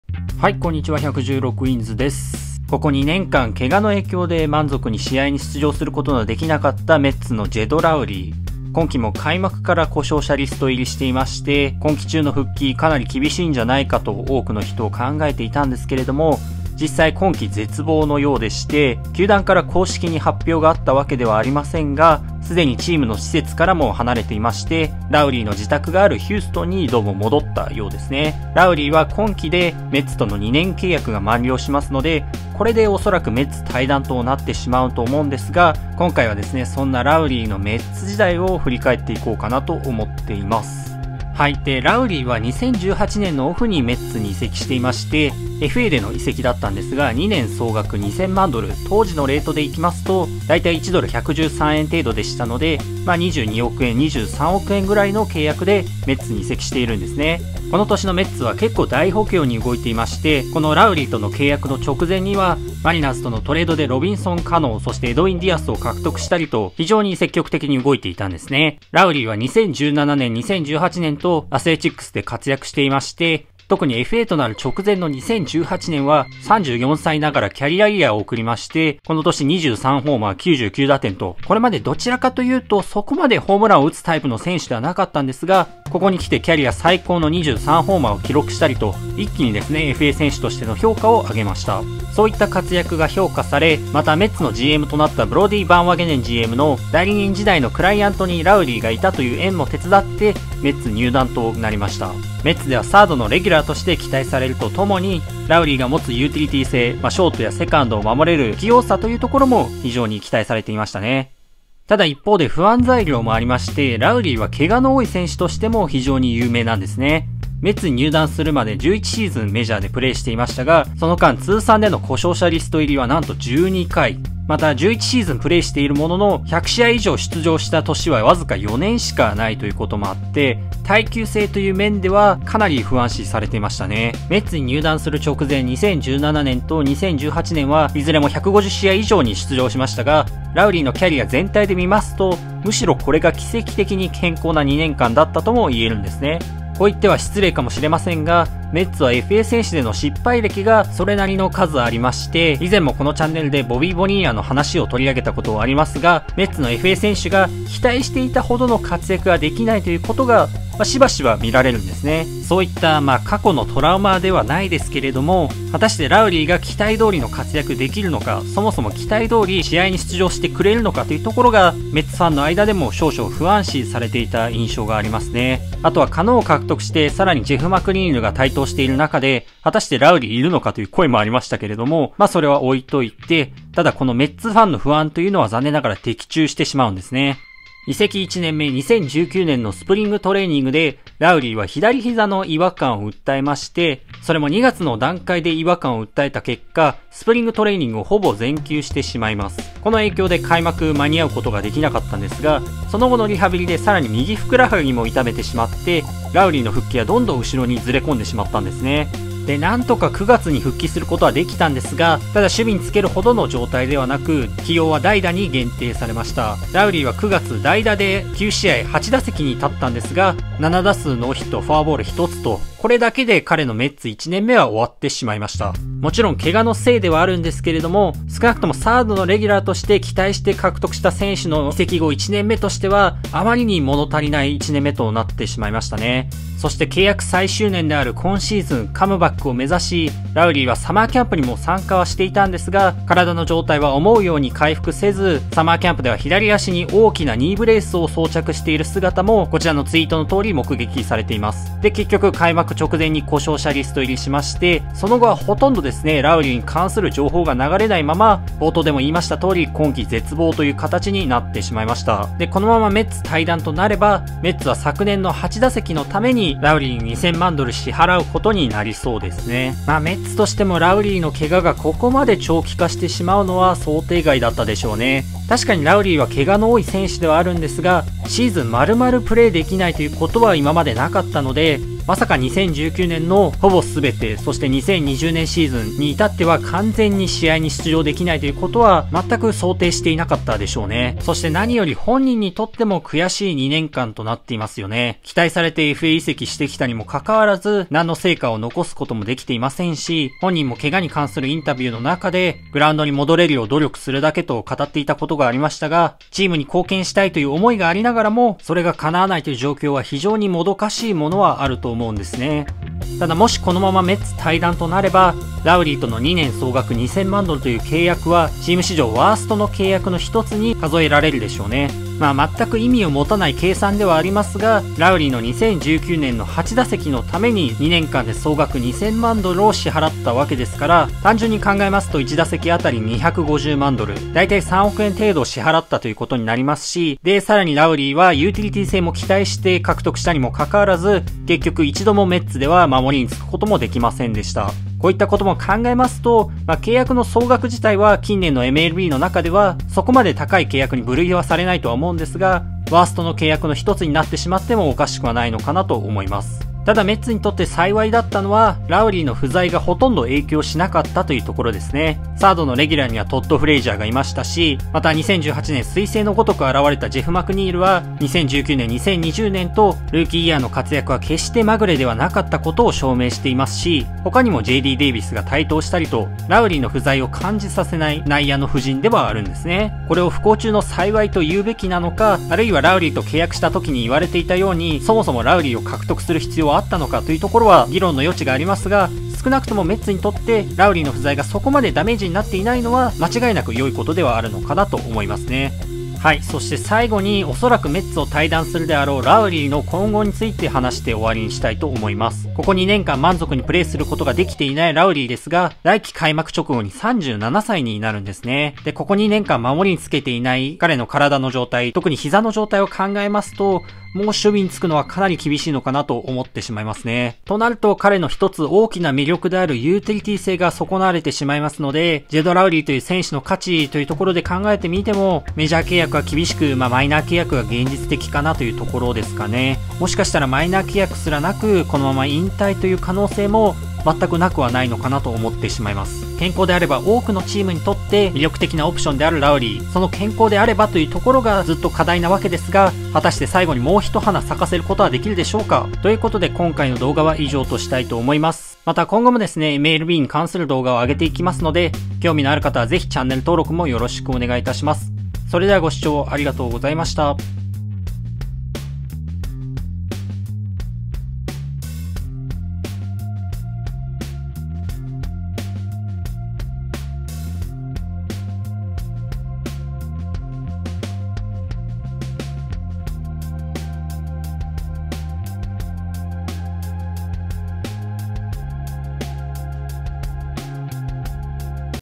【MLB】2年総額約22億円の契約も8打席でメッツ退団しそうなジェド・ラウリー
ジェド・ラウリーの失望のメッツ時代が終わりそうです。

2018年オフに2年総額2000万ドルの契約でメッツに加入しましたが、怪我の影響で昨年は8打席にしか立てず、今季は1試合も出場することなくシーズンを終えることになりそうです。

どうしてこうなった…

■メッツ史上ワースト契約（と思われる）ボビー・ボニーヤ